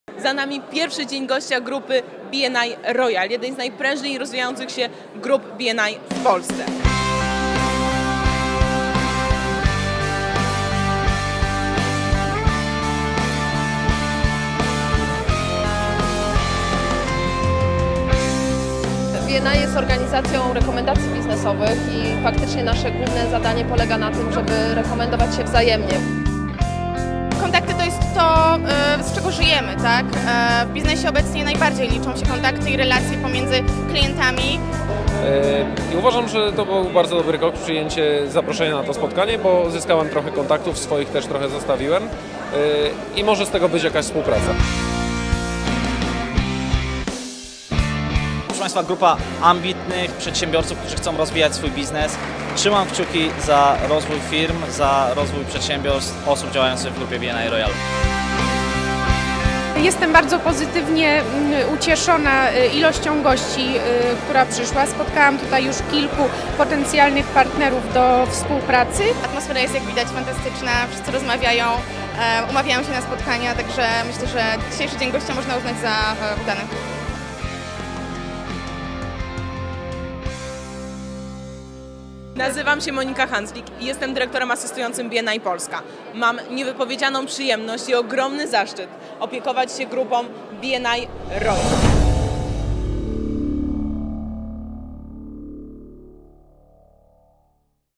BNI ROYAL Dzień gościa 2014
Dziękujemy wszystkim przedsiębiorcom,
którzy w dniu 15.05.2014r. odpowiedzieli na nasze zaproszenie i chcieli razem z grupa BNI ROYAL rekomendować biznes podczas
Dnia Gościa BNI ROYAL W centrum Konferencyjnym WItek.